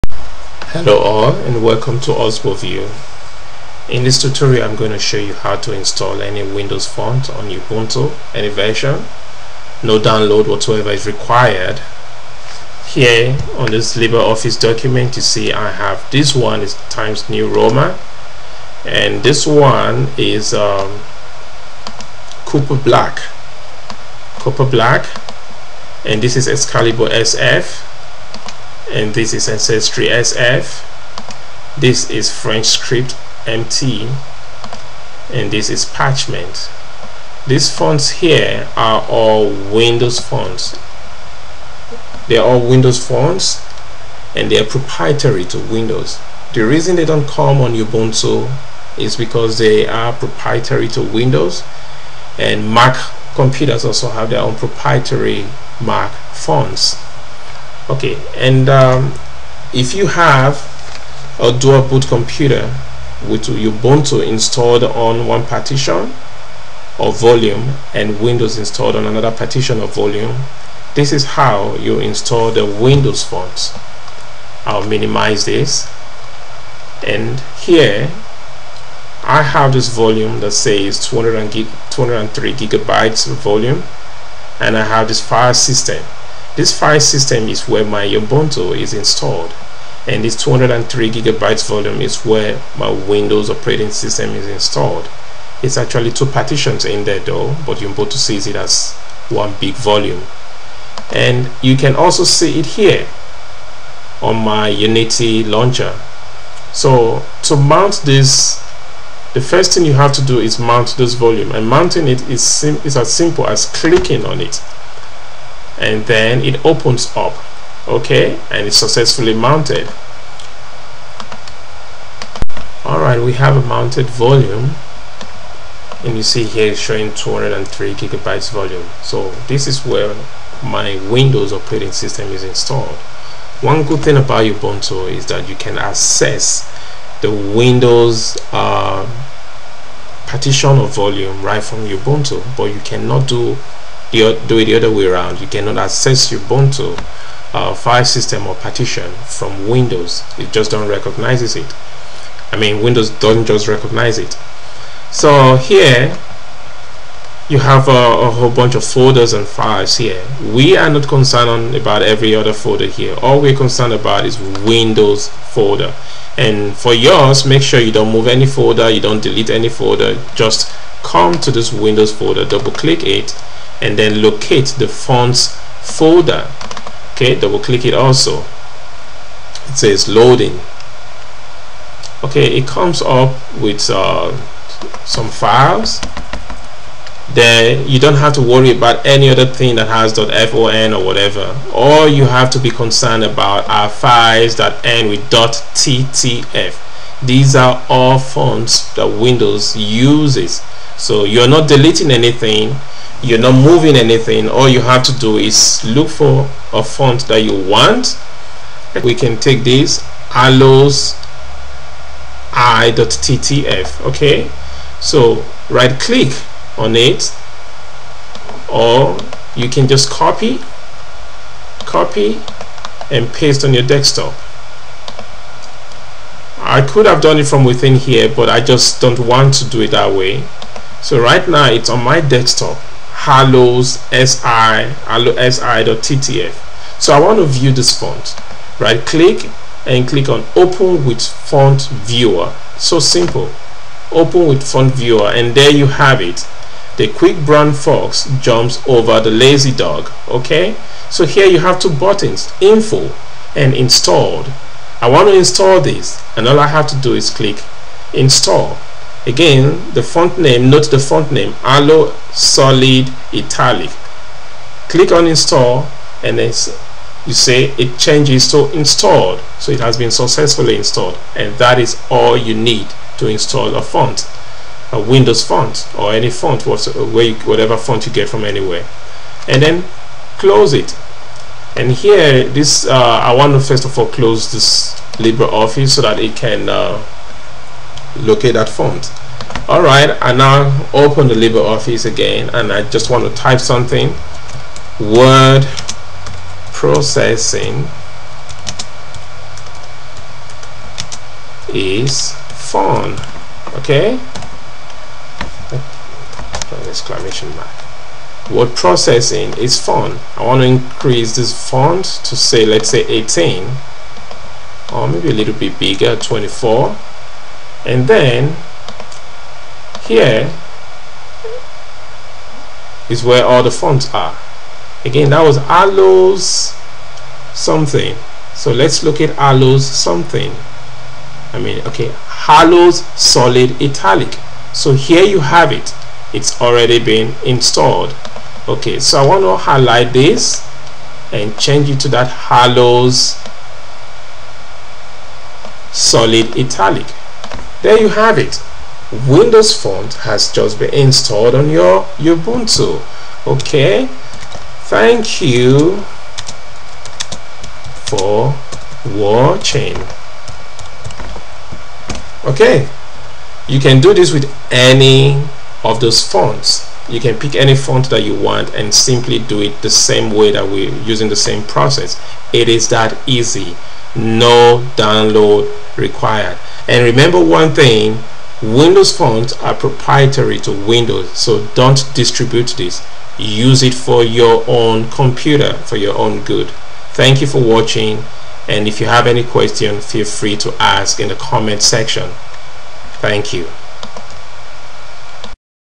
How to Install Any Microsoft Windows Font on Ubuntu X.X - No Download Required
How to Install Any Windows Font on UBUNTU X.X -- No Download Required

If you have a dual boot computer with Ubuntu installed on one partition or volume, and Windows installed in another partition or volume, you can install your Windows fonts on Ubuntu easily without any download. These fonts can be used on LibreOffice, OpenOffice, or anything that requires the use of  fonts. This is how you do it:

1. Mount your Windows partition or volume by clicking on it

2. On the opened window containing folders and files, locate the Windows folder

3. Double-click on the Windows folder and locate the Fonts folder

4. Double-click on the Fonts folder and focus on the files with .ttf or .TTF extension

5. Select any font of your interest, copy and paste on your Ubuntu desktop or any location of your choice. ( I just created a folder named Fonts on my USB flash drive and named it Fonts, then copied and paste any font to that folder). Note: You can also continue directly on the Windows/Fonts folder without copying the file.

6. Right click on the copied file and select Open With Font Viewer.

7. The Font Viewer opens for you to preview the font. Note the font's name, and at the top-right corner, you have two buttons name Info and Install. Click on Install. The install button will change to Installed after about 2 seconds.

8. Open LibreOffice or OpenOffice and test your newly installed font.

9. Thank you!